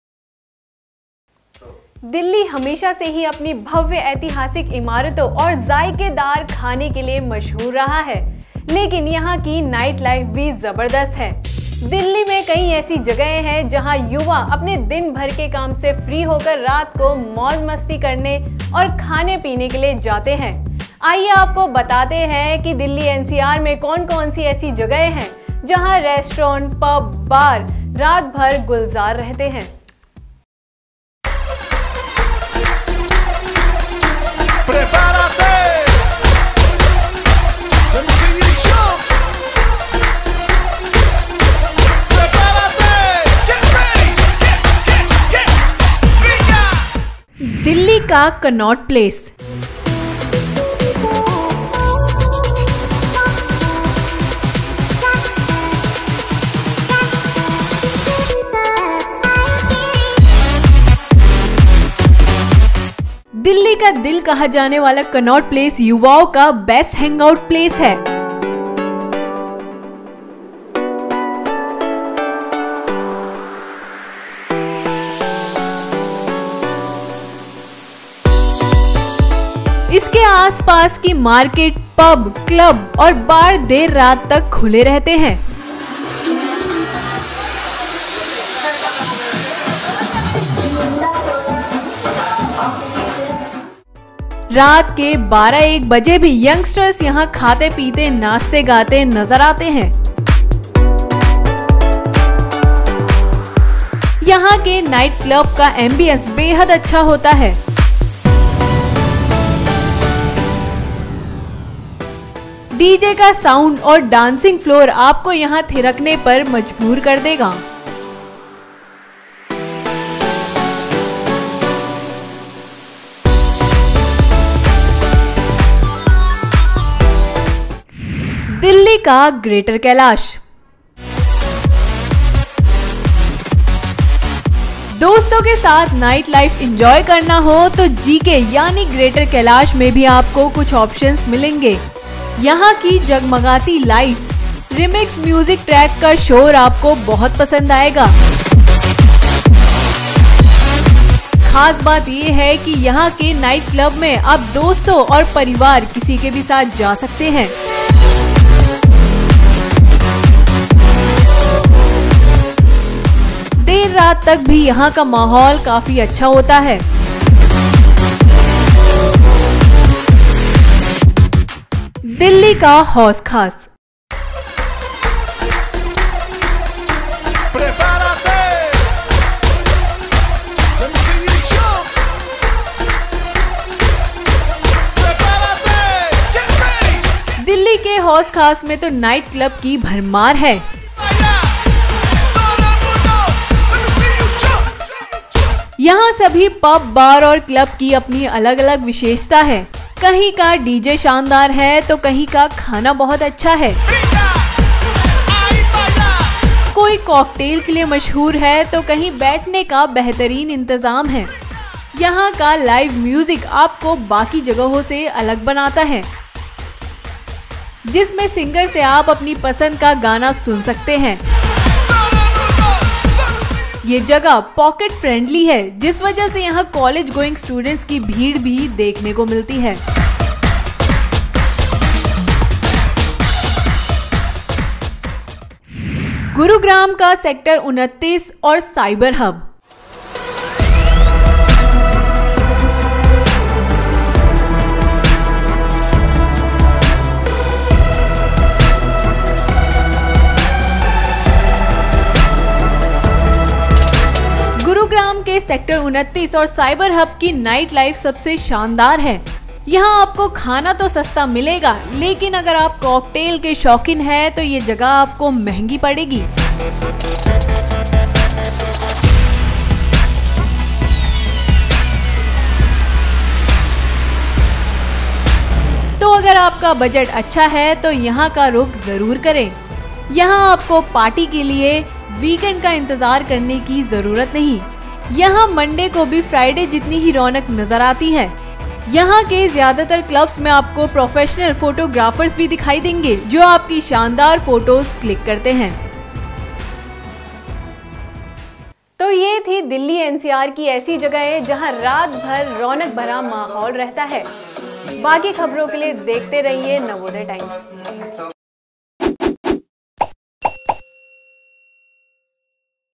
Delhi में 31st Night Celebrate करना है तो इन जानें-मानी जगहों पर कर सकते हैं रूख | Happy New Year
अगर आप 31 दिसंबर की रात सेलिब्रेटकरने बाहर जाना चाहते हैं तो दिल्ली में इन जगहों का रूख कर सकते हैं। दिल्ली में कई जगहों पर रेस्तरां, बार, डिस्को और पब रात भर गुलजार रहते हैं।